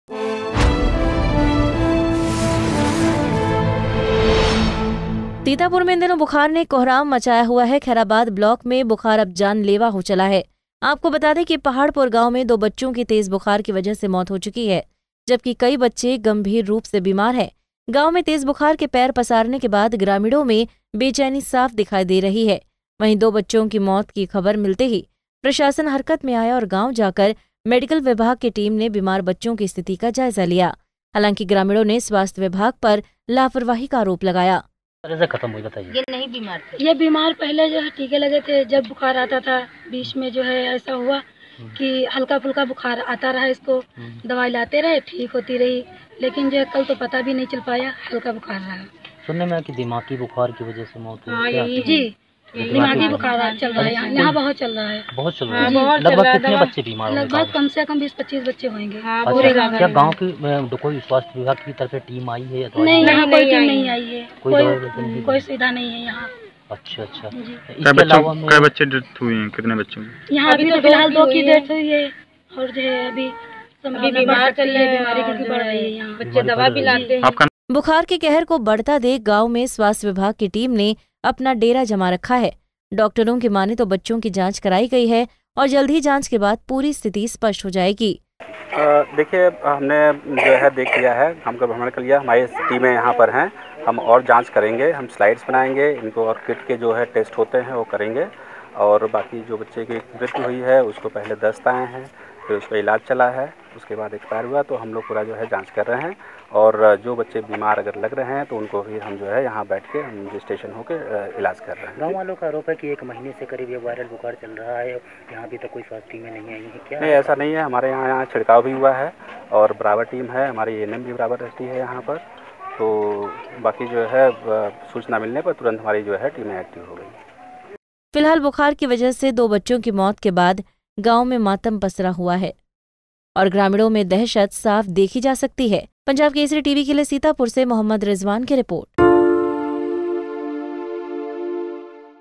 जानलेवा हुआ बुखार, 2 बच्चों की मौत, कई बीमार
सीतापुर में इन दिनों बुखार ने कोहराम मचाया हुआ है...खैराबाद ब्लॉक में बुखर अब जानलेबा हो चला है..आपको बता दें कि पहाड़पुर गांव में 2 बच्चों की तेज बुखार की वजह से मौत हो चुकी है जबकि कई बच्चे गंभीर रुप से बीमार हैं..